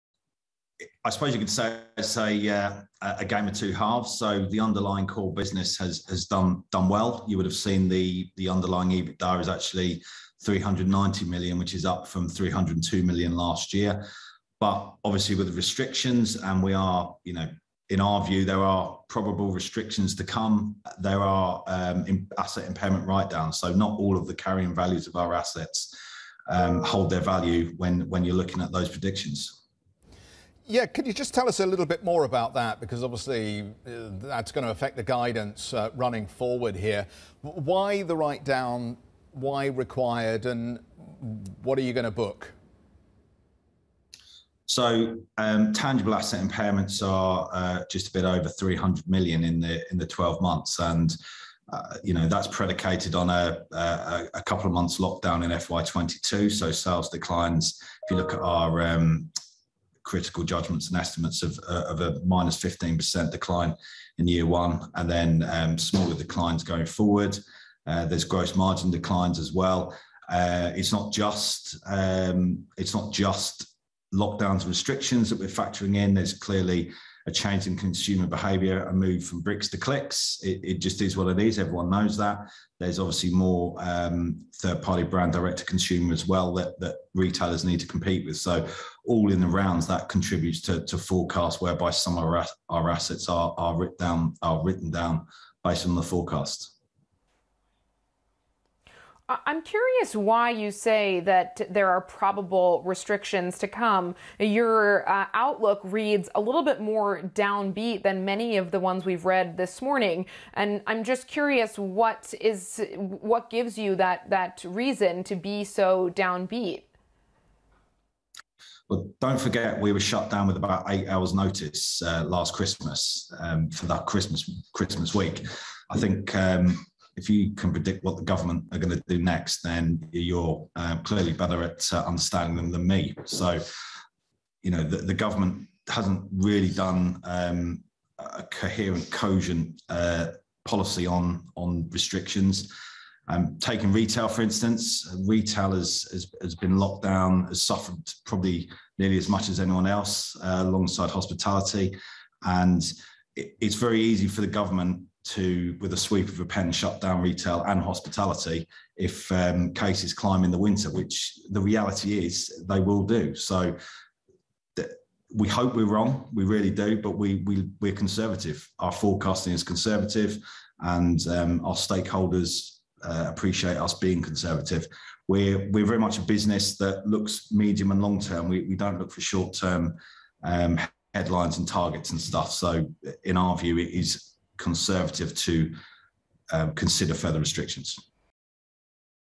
Frasers Group maintains conservative forecast: 'Probable restrictions to come,' says CFO
Frasers Group CFO Chris Wootton discusses the British retail company's half-year results, preparing for a potential downbeat forecast amid growing variant cases and unpredictable government guidelines.